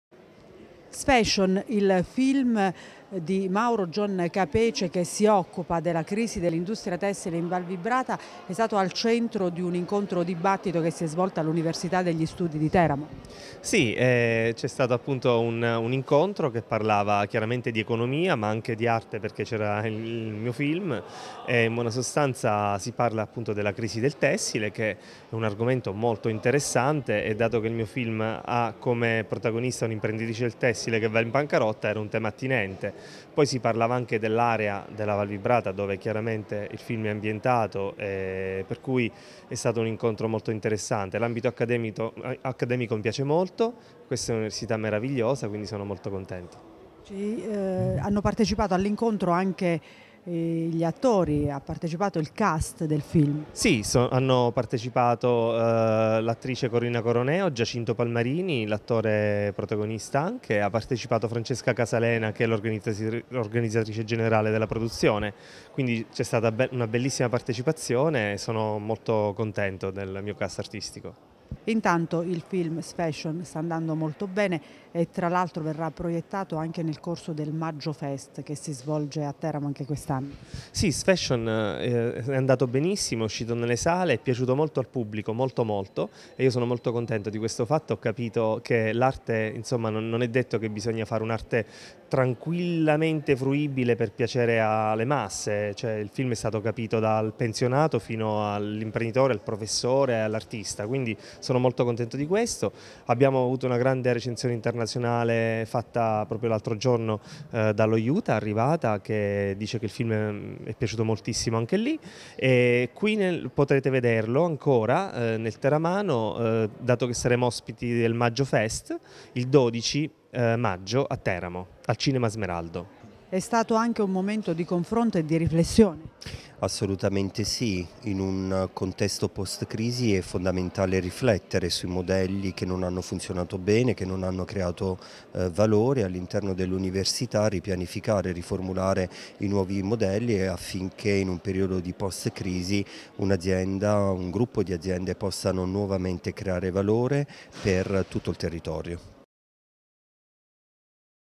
UNIVERSITÀ, INCONTRO CON IL CAST DEL FILM SFASHION SULLA CRISI DELL'INDUSTRIA TESSILE IN VAL VIBRATA
La crisi dell'industria tessile in Val Vibrata. Se ne è discusso all'Università di Teramo con il cast del film "Sfashion".

Servizio di J News - 19 aprile 2017